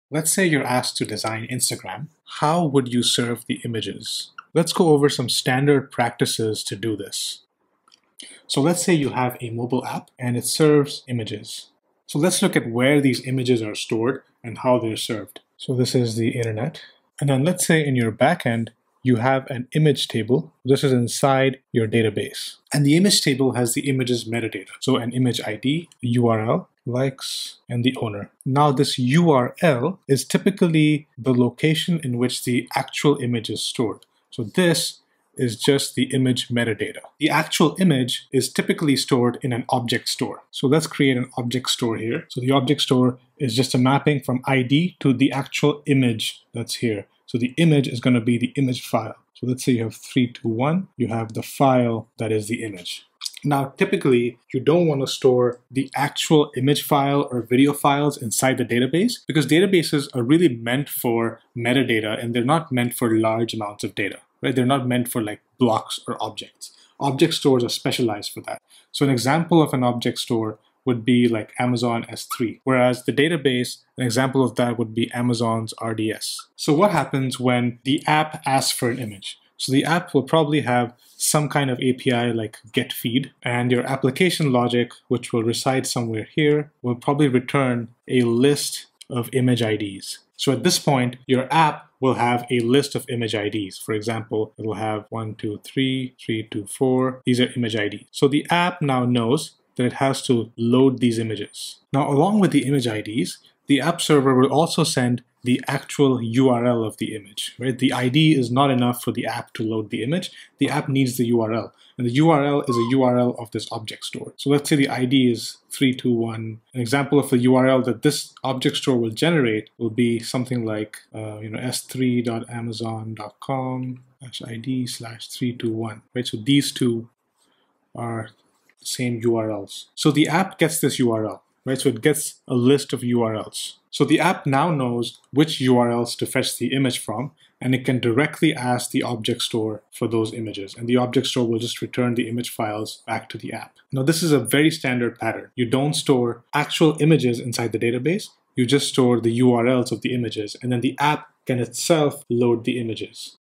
Ep 11 - How Instagram Stores Images (Part 1 - Storage)
In System Design Daily, we cover system design topics for technical interviews.